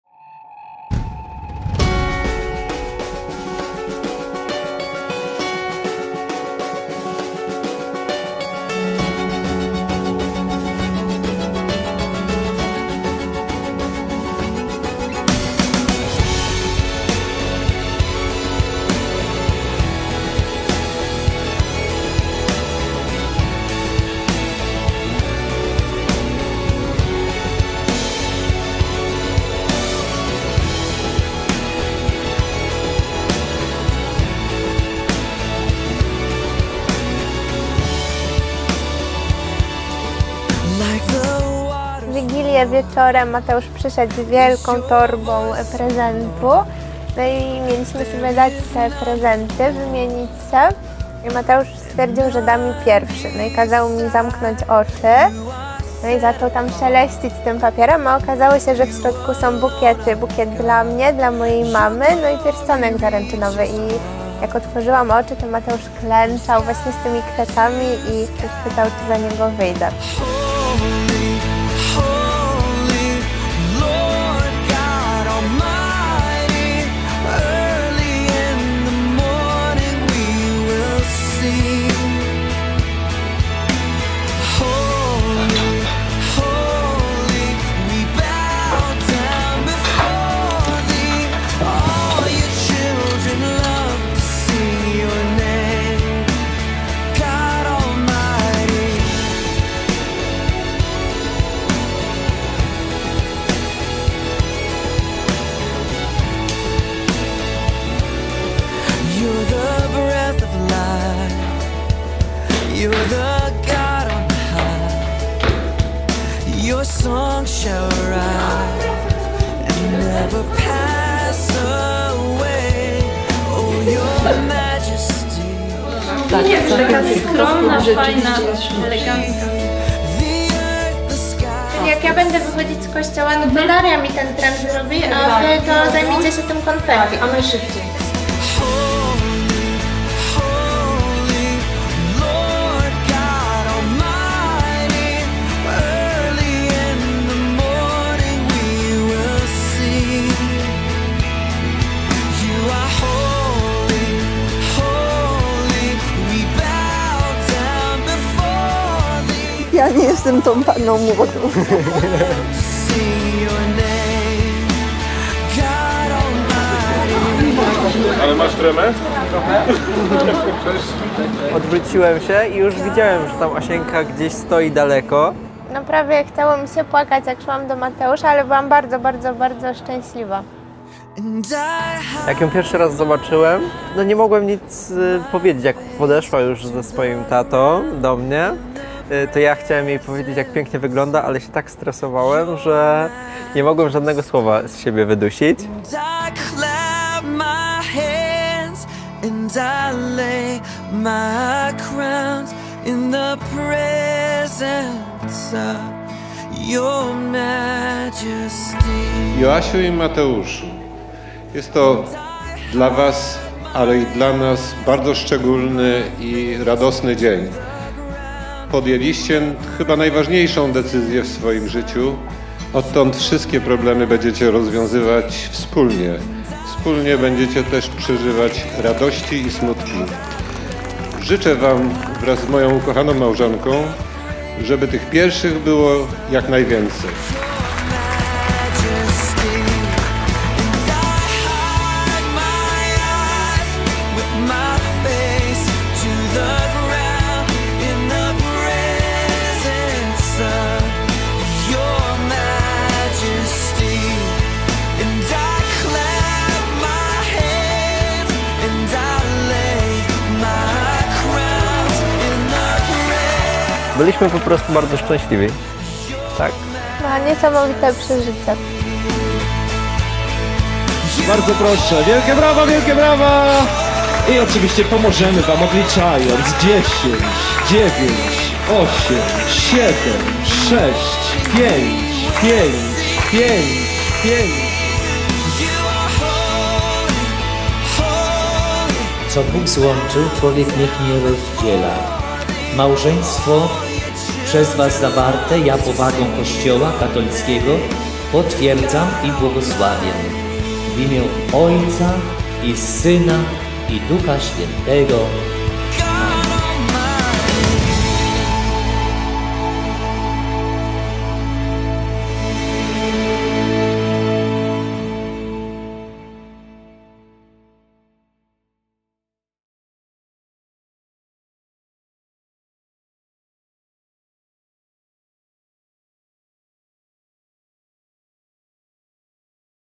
Wyjątkowe Wesele Asi i Mateusza w Pałacu Chojnata | Kamerzysta Rawa Mazowiecka
Zapraszamy Was na niezwykłą podróż w świat miłości i elegancji na wesele Asi i Mateusza, które odbyło się we wspaniałym miejscu, jakim jest Pałac Chojnata.

Ceremonia ślubu odbyła się w malowniczym Kościele św. Wawrzyńca Diakona i Męczennika w Wilkowie Pierwszym, a sesję plenerową po ślubie zrobiliśmy na Zamku w Gołuchowie oraz w Kaliszu.

To był dzień pełen elegancji, wzruszeń i ciepłych słów, który Asia z Mateuszem i ich bliskimi będą wspominać przez wiele lat!

Skontaktuj się z nami już teraz, aby zarezerwować termin na Twój wyjątkowy dzień!

Kamerzysta Rawa Mazowiecka